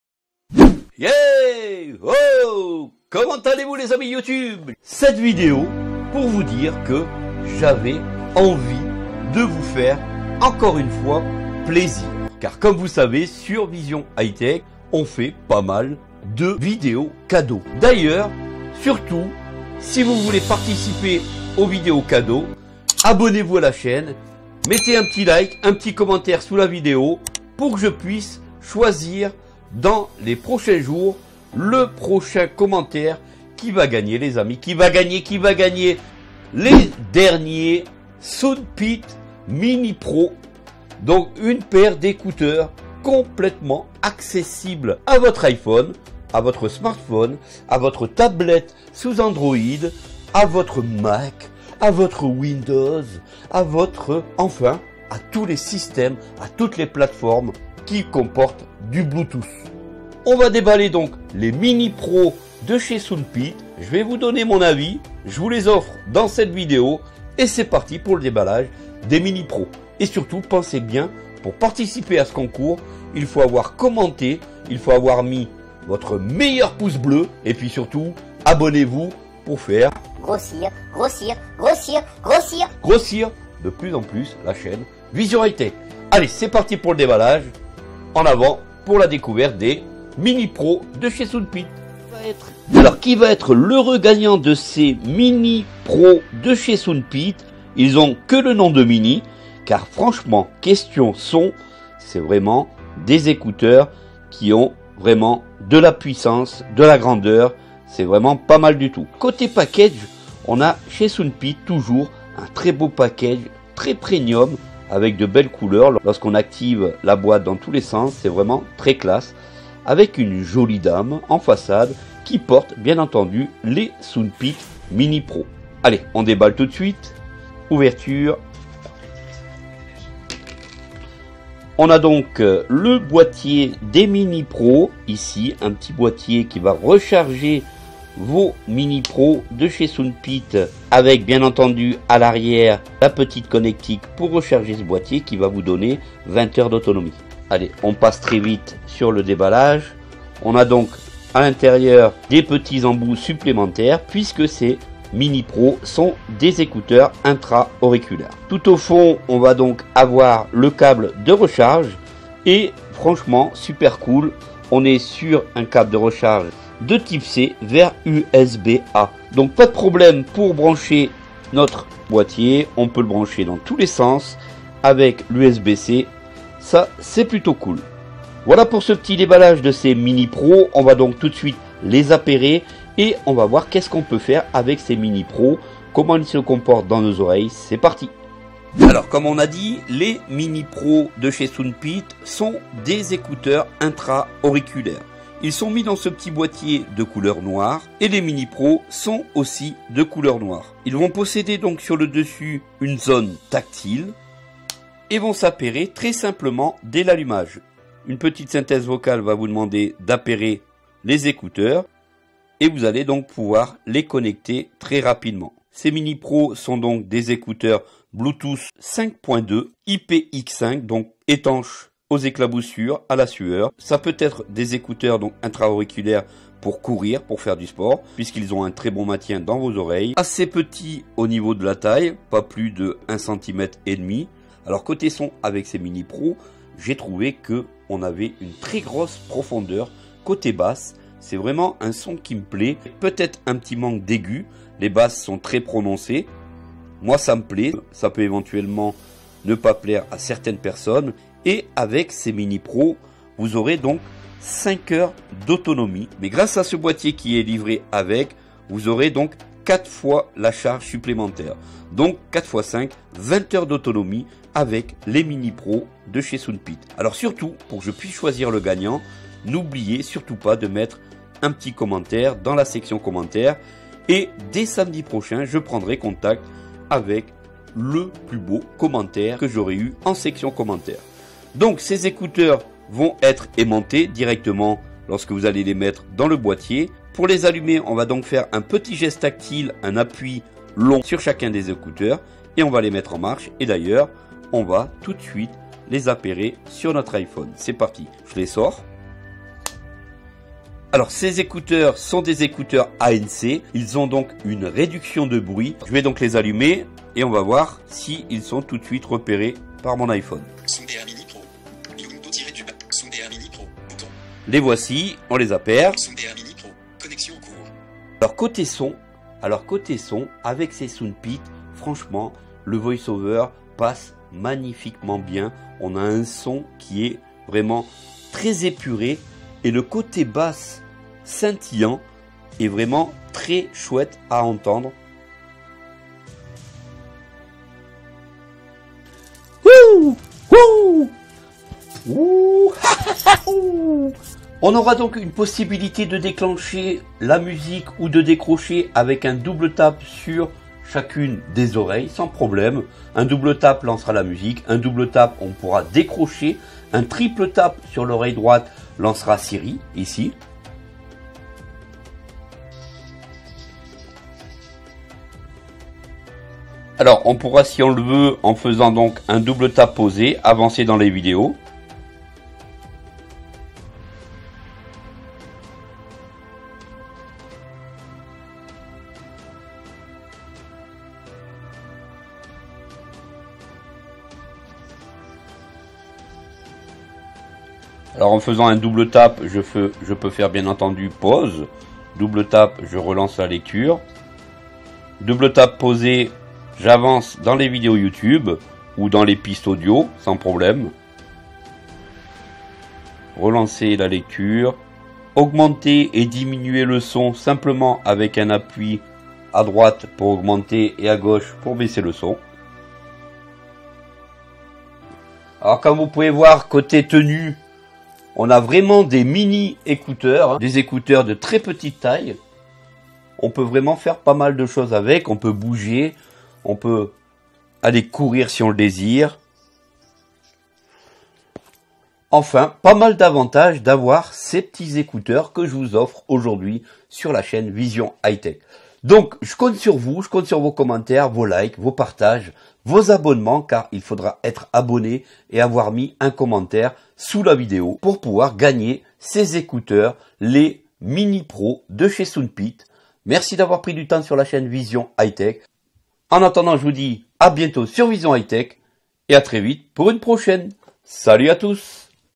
Test Mini Pro SoundPeats : GAGNEZ vos mini Pro compatible iOS iPhone Android Windows Et Mac
J'ai testé les Mini Pro de chez SoundPEATS, je les ai trouvé incroyable et j'ai eu envie de vous les faire gagner que pensez-vous de ce petit cadeau.
Vous allez vraiment aimer ce cadeau compatible iOS, Android, Windows et Mac ne ratez plus aucun podcast, aucune musique ou aucune vidéo avec un sont magnifiques.

voici le lien de Mini Pro

Et d'ailleurs, c'est le code 5% : KH6XUMUI.